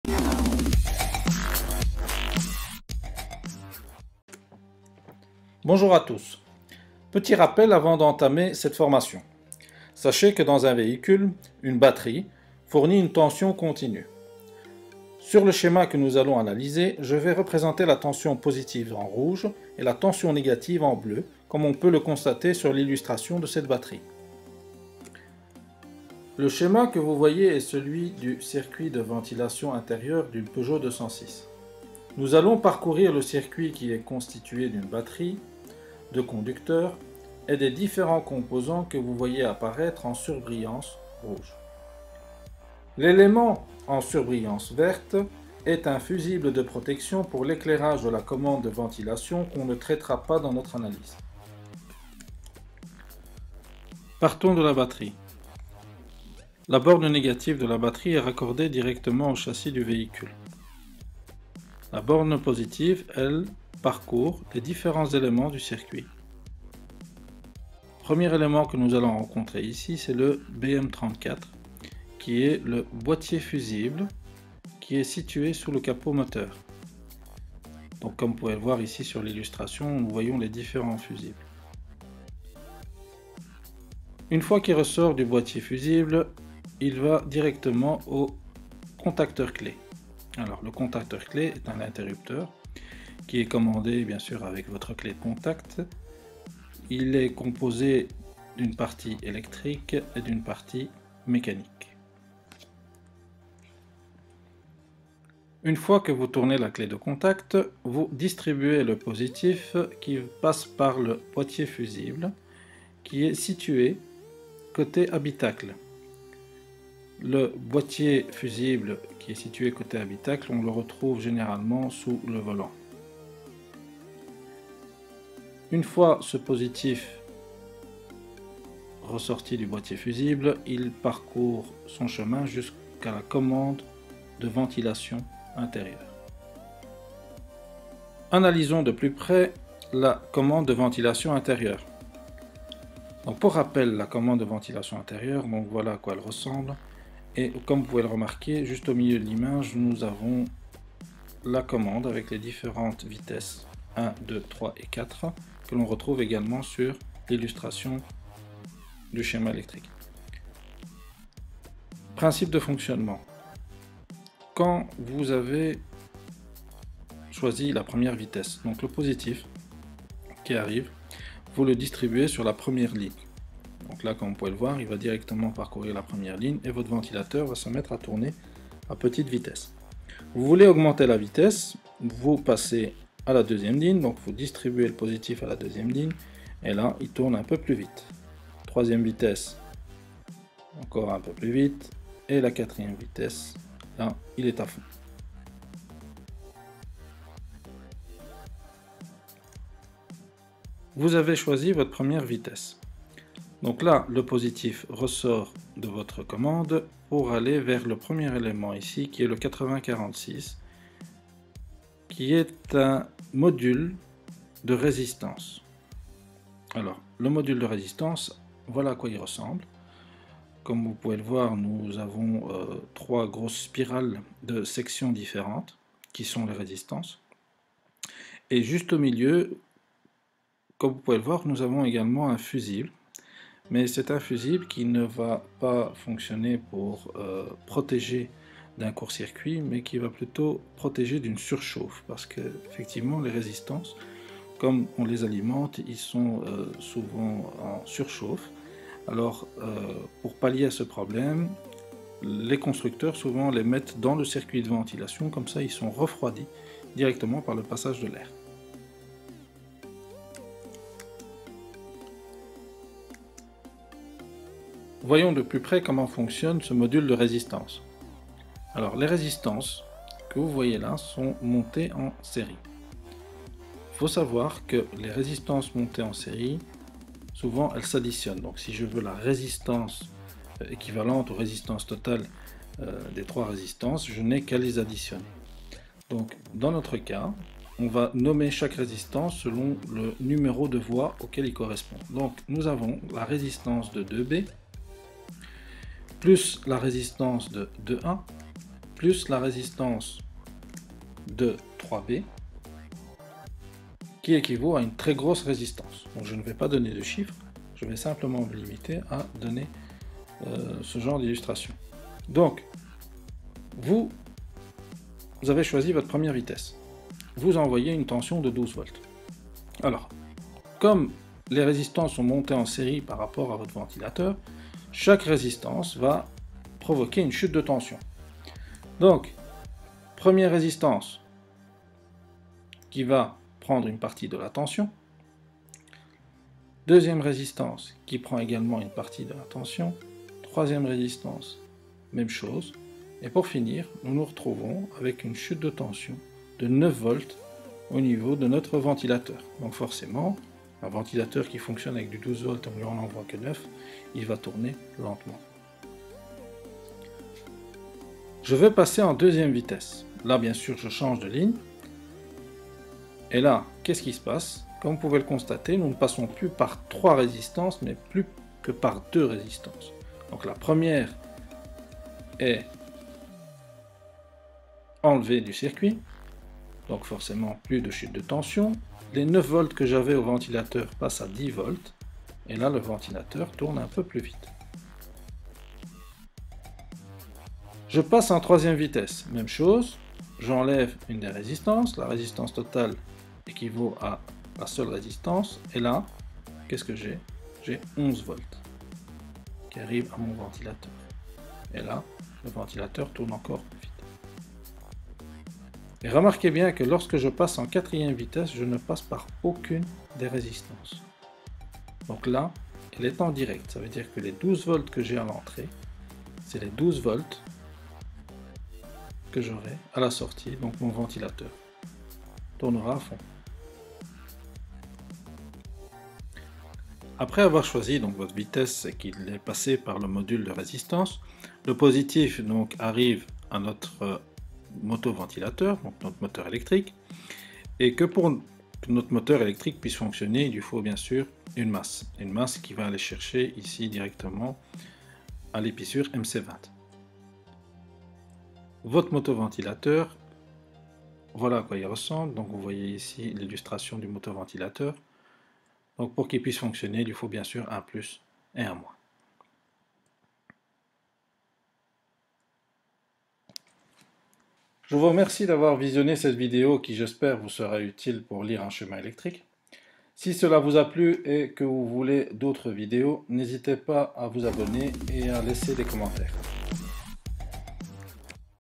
Lecture Schéma Ventilation
Formation pour apprendre à analyser un schéma électrique d'un véhicule